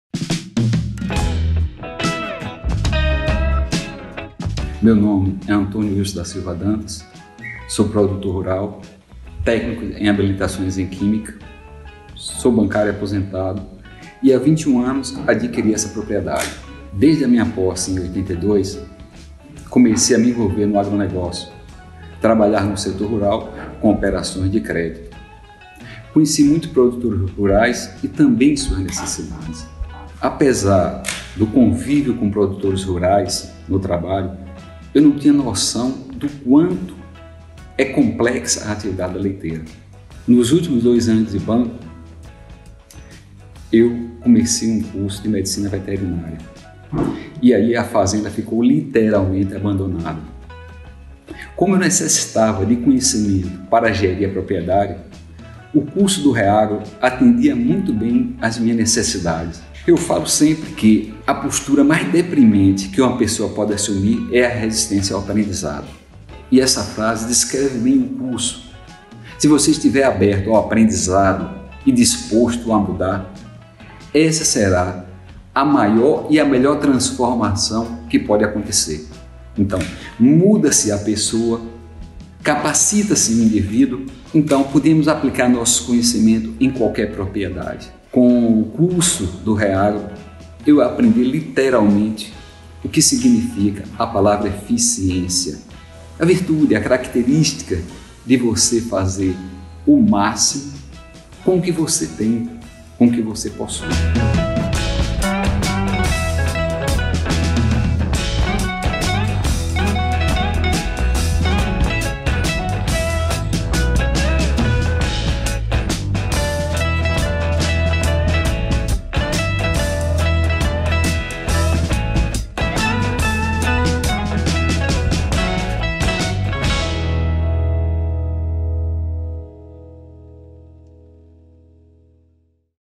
CONHECIMENTO que gera TRANSFORMAÇÃO | De Olho no Futuro
Ao longo desses 19 anos, nos dedicamos em consolidar nosso propósito: transformar vidas através do agronegócio!

Você confere, no 1° episódio da série De Olho no Futuro, o depoimento do nosso ex-aluno Antônio Wilson, mostrando como o curso de Gestão na Pecuária Leiteira tornou muito mais eficiente a gestão de seus negócios.

Acesse o nosso site e conheça mais sobre o curso de Gestão na Pecuária Leiteira